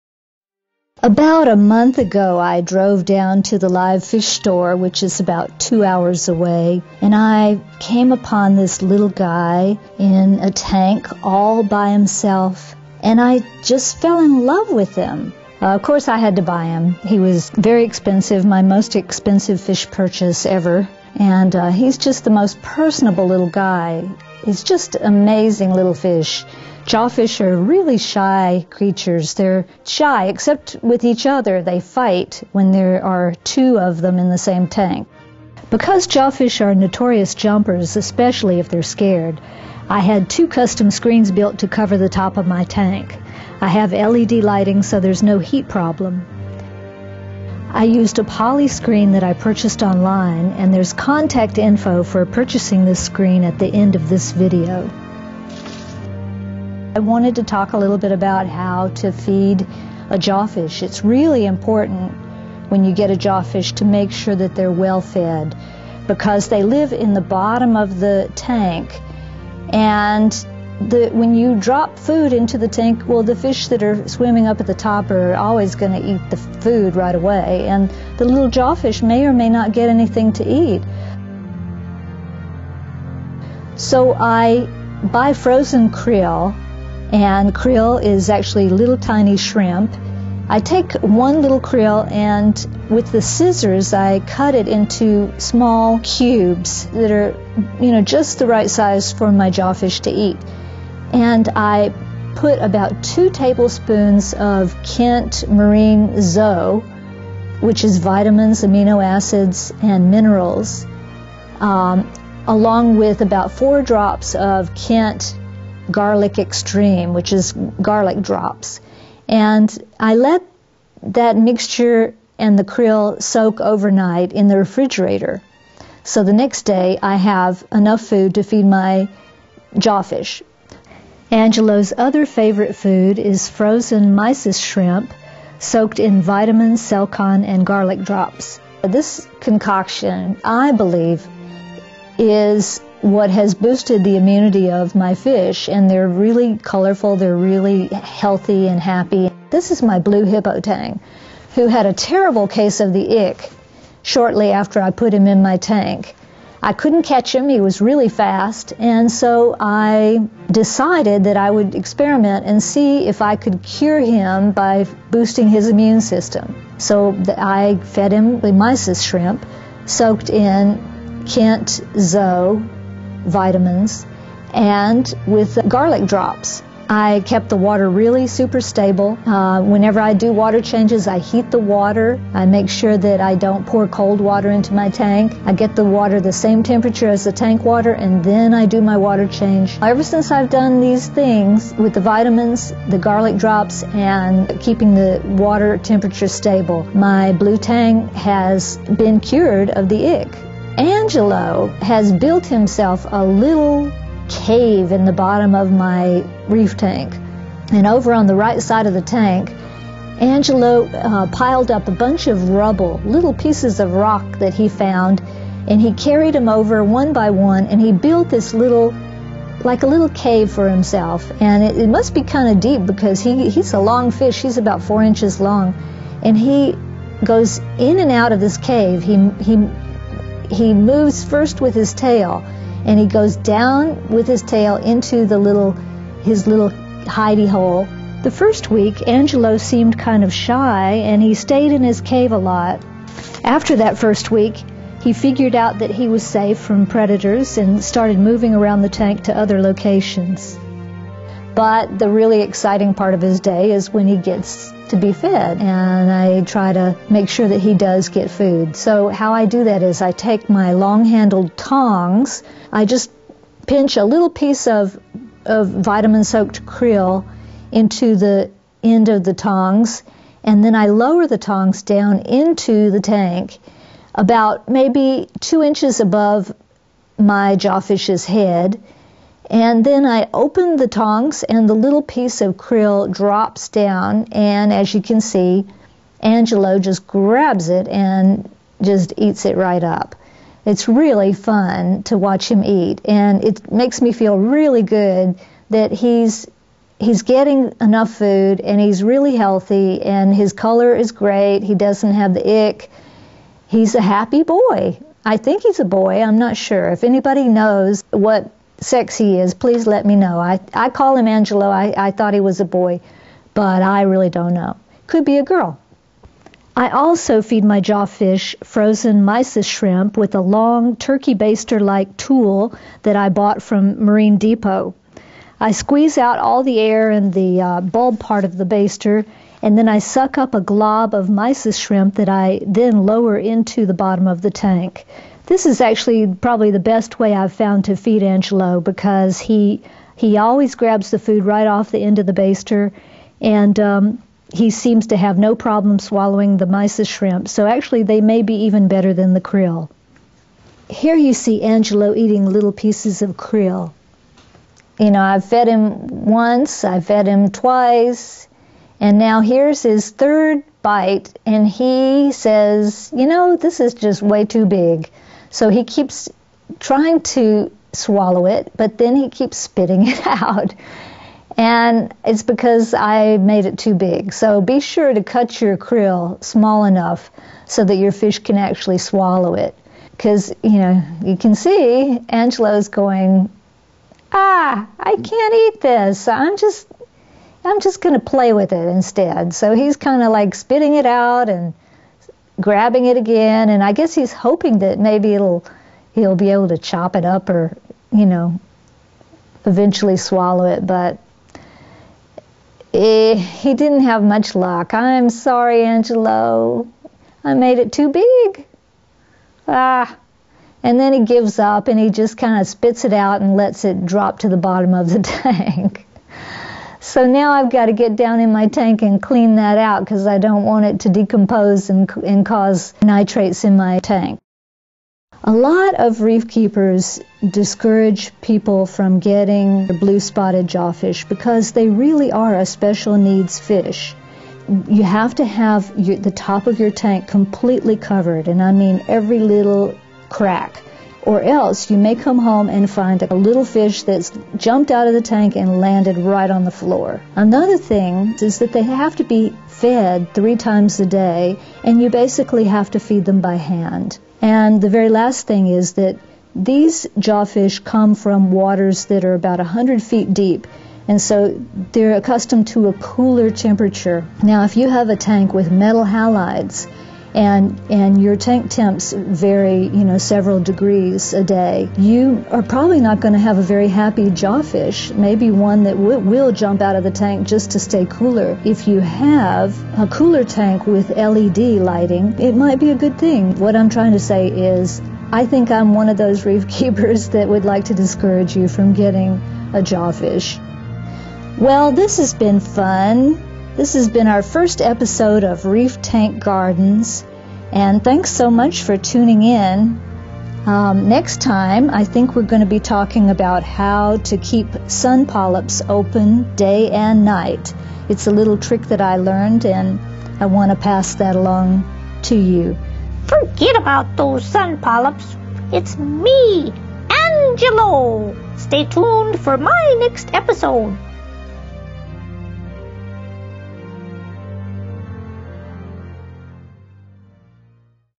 Angelo, The Blue-Spotted Jawfish
The Blue-Spotted Jawfish is a "special needs" fish in the marine aquarium.  This video introduces this delightful, curious, and personable little fish and explains what it takes to keep the Blue-Spotted Jawfish healthy, happy, and content in a salt water tank.